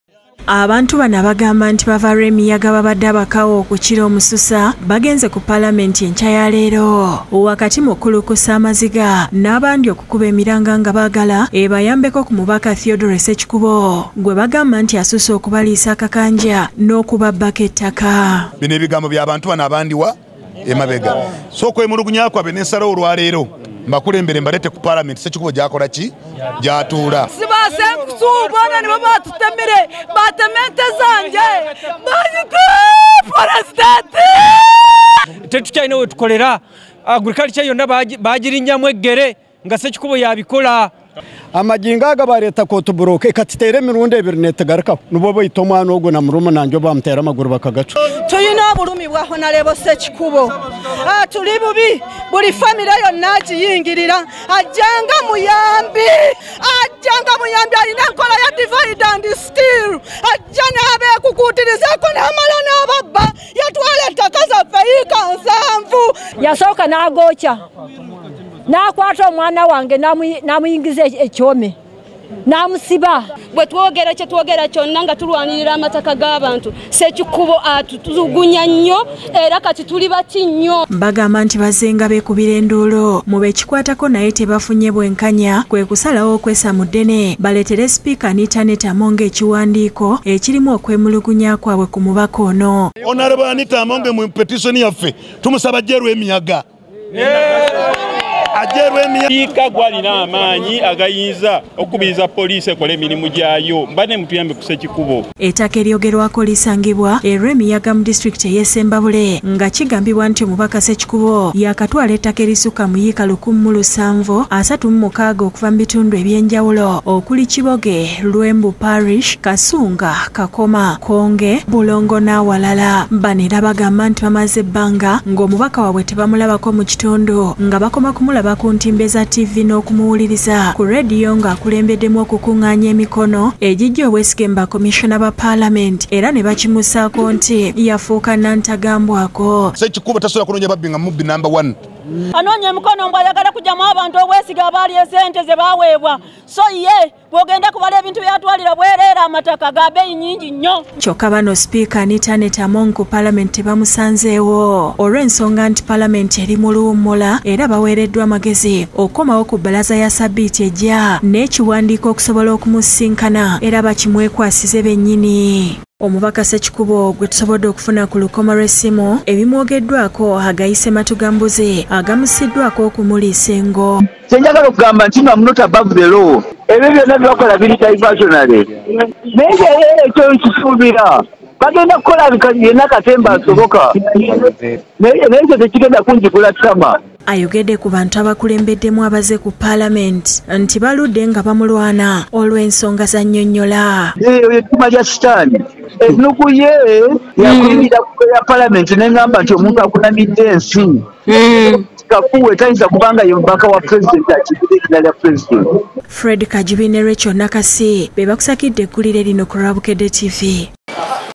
Abalumiriza Omubaka Ssekikubo okubabbira ettaka n'okubatulugunya baddukidde ku Palamenti
Eby'omubaka we lwemiyaga Theodore Ssekikubo byongedde okumwononekera oluvannyuma lwa batuuze okuva mu kitundu gyakiikirira ekye lwemiyaga okweyiwa ku palamenti mu ttuntu lyaleero wakati mu kukulukusa amaziga kko nokukuba emiranga  ngabaagala Palamenti ebayambeko kumubaka wabwe asusse okubaliisa akakanja nokubabbako ebyobugagga bwabwe.